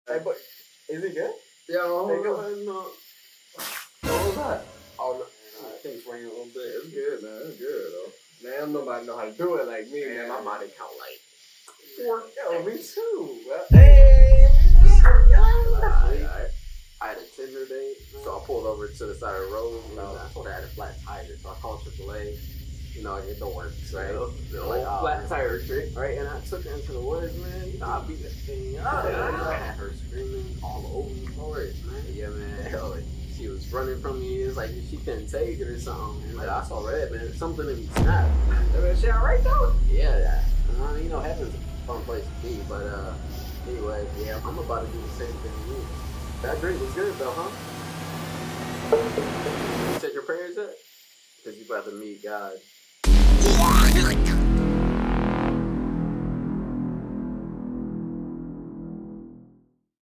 When the creepy friend talks about body count
Do you usually clarify what you mean about body count? Let's stay safe out there😂

🕘Timestamps:
0:00 Spiking the drink
0:13 Body count easter egg
0:30 When it gets sketchy
0:49 Getting tased

SOCIAL